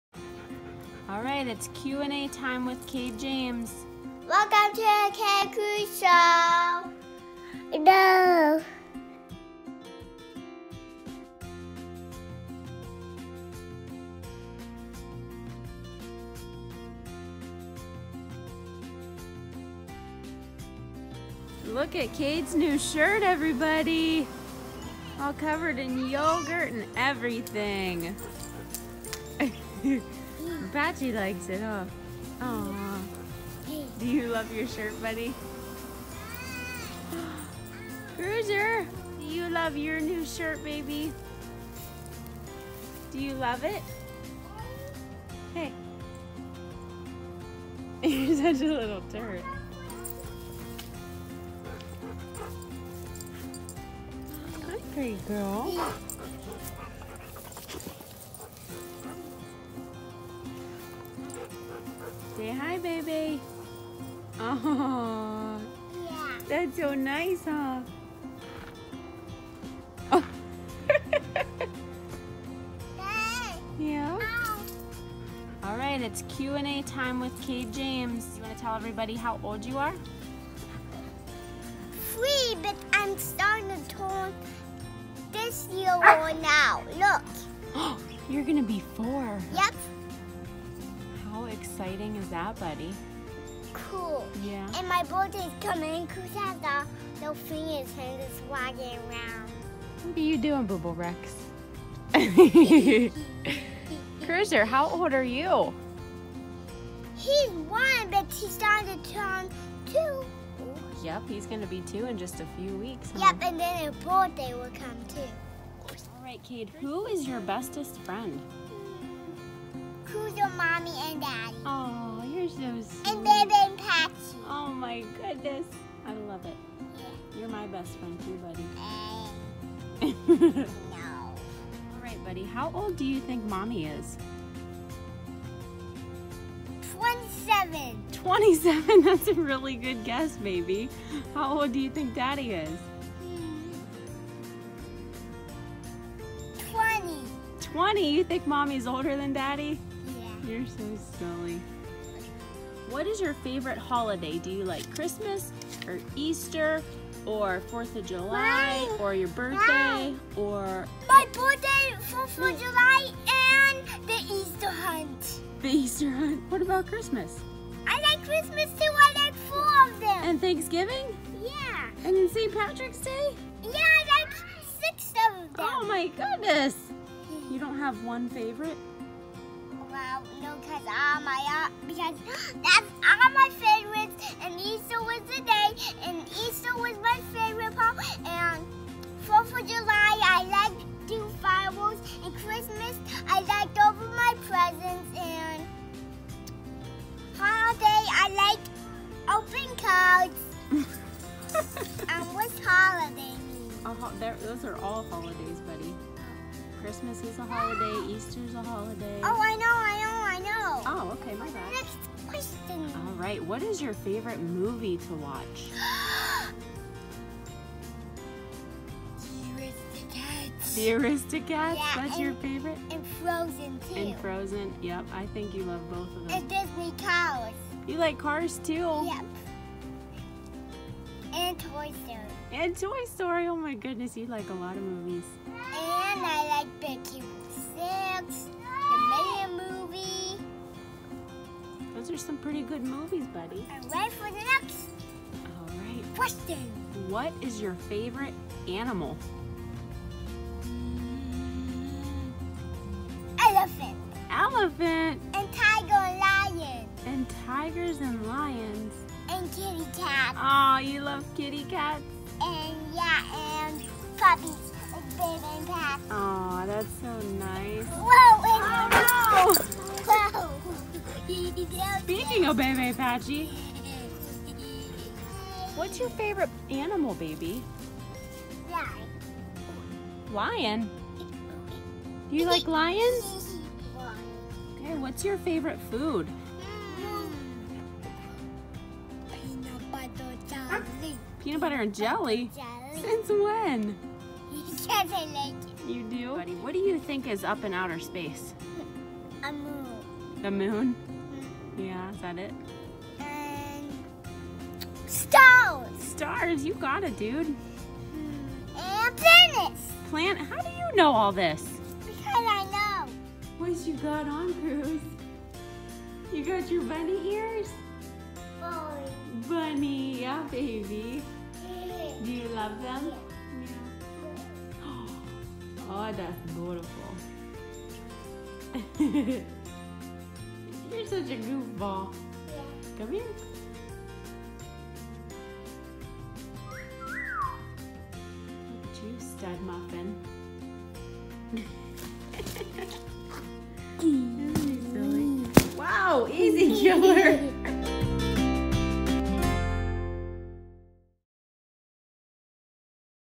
Q&A with Cade!
It’s time to find out some silly things about Cade in our 1st ever Q&A!! He absolutely loved doing this questions and answers video and I foresee more of these in the future?! :) Cruz made his appearances few time with his adorable, goofy ways and the huskies snuck in some love from the boys too! We hope you enjoy hearing Cades responses to my questions!!